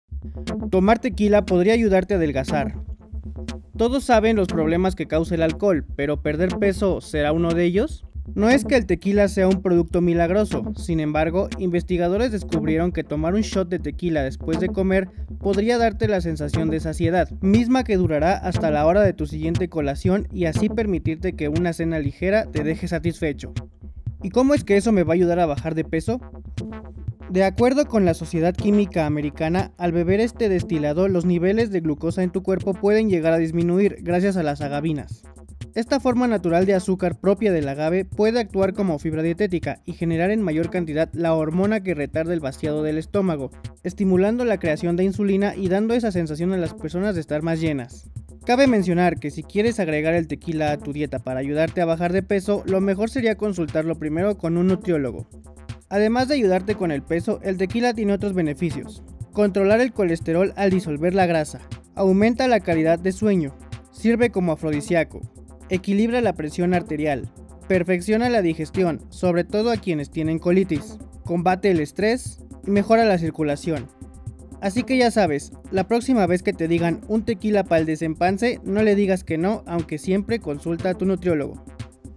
¿Buscas adelgazar? Tomar tequila podría ayudarte a bajar de peso
No es que el tequila sea un producto milagroso, sin embargo, investigadores descubrieron que tomar un 'shot' de tequila después de comer podría darte la sensación de saciedad, misma que durará hasta la hora de tu siguiente colación y así permitirte que una cena ligera te deje satisfecho.

Grupo Fórmula